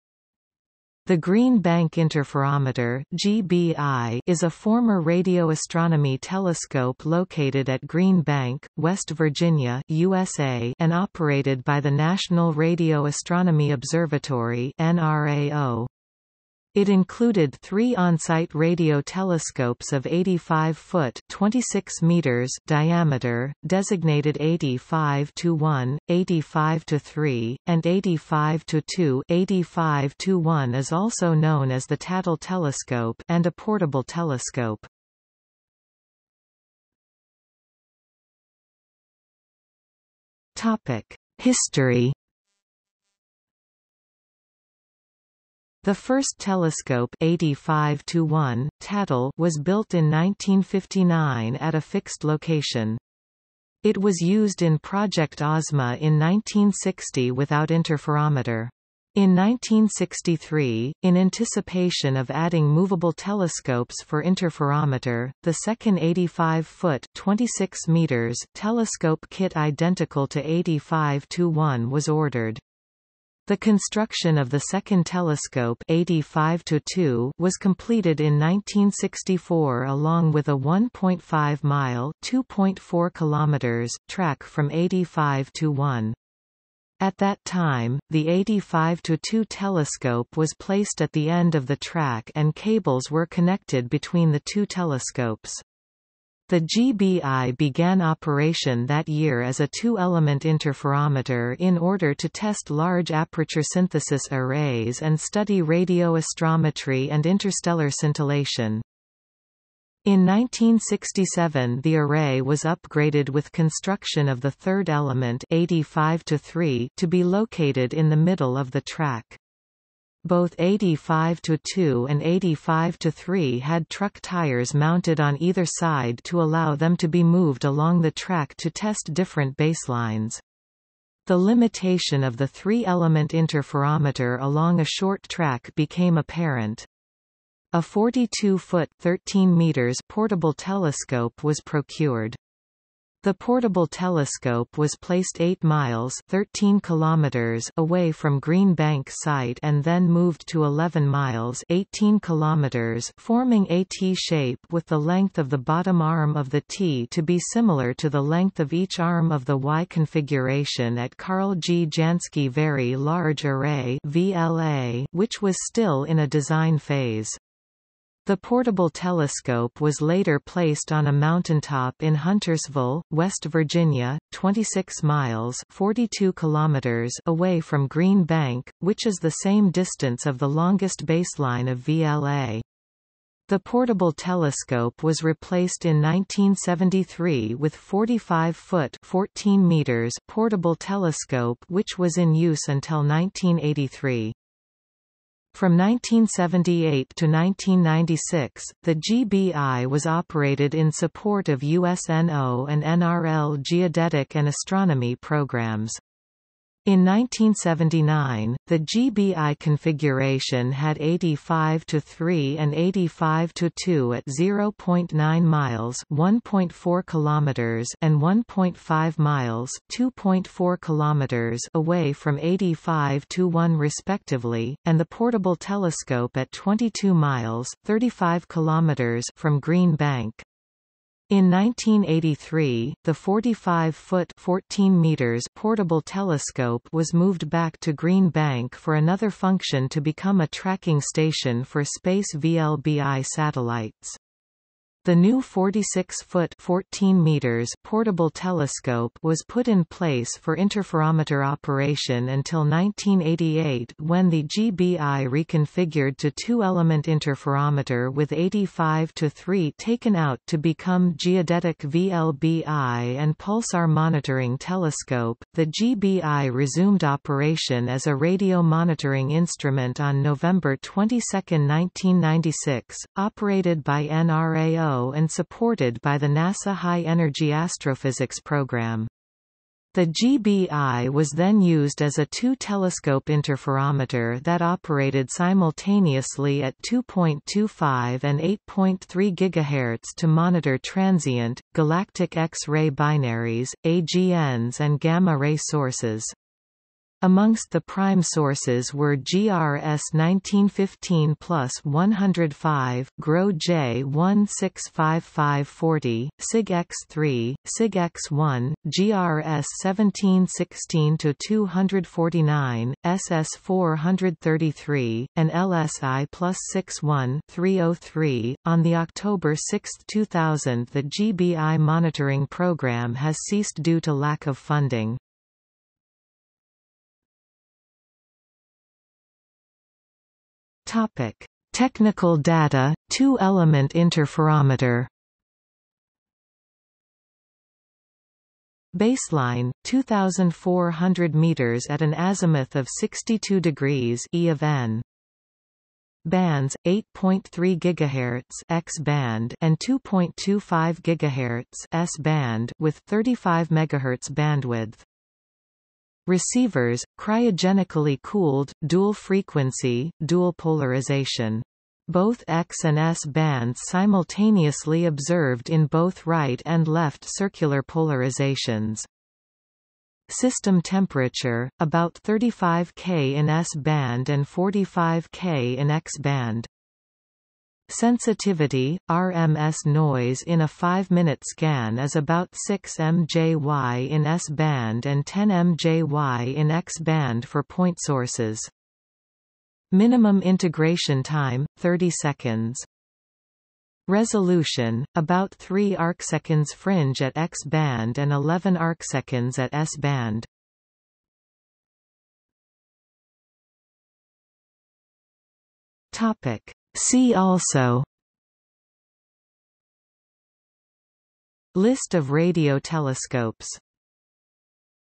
Green Bank Interferometer | Wikipedia audio article
00:00:36 1 History
00:05:27 2 Technical Data: 2-element interferometer
00:06:42 3 See also

- Socrates

SUMMARY
The Green Bank Interferometer (GBI) is a former radio astronomy telescope located at Green Bank, West Virginia (USA) and operated by the National Radio Astronomy Observatory (NRAO). It included three on-site radio telescopes of 85-foot (26m) diameter, designated  85-1, 85-3, and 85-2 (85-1 is also known as the Tatel Telescope) and a portable telescope.